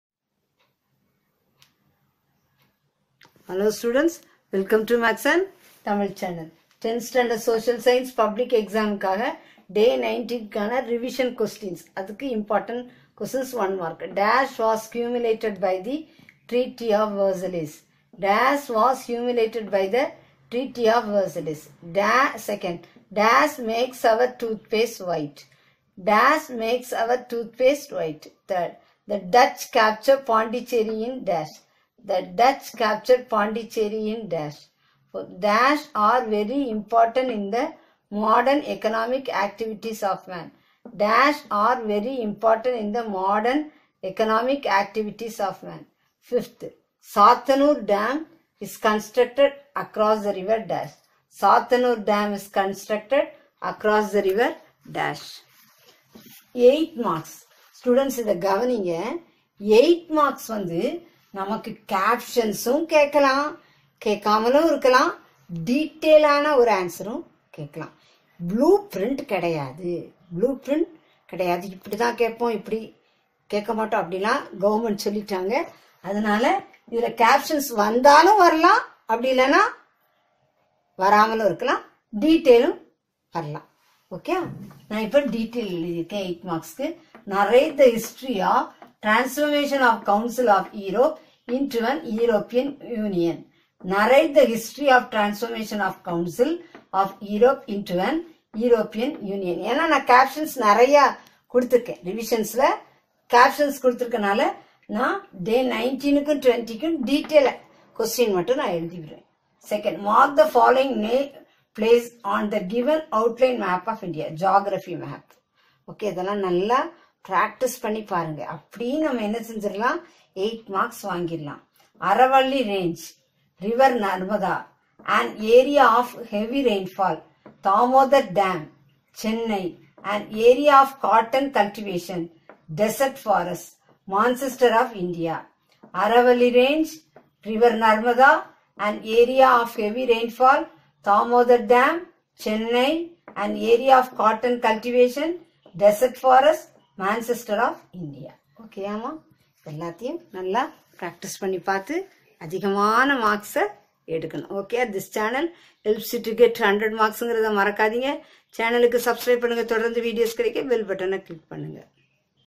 T.N. 10th std Social Science Public Exam | Important 1 & 8 Mark | Revision Questions Day -19.
Dear Students,
This video Tells about that 10th std Social Science
Public Exam
 score high marks
 Important 1, & 8 marks Revision Questions
This channel helps you to get 100 marks in Social Science.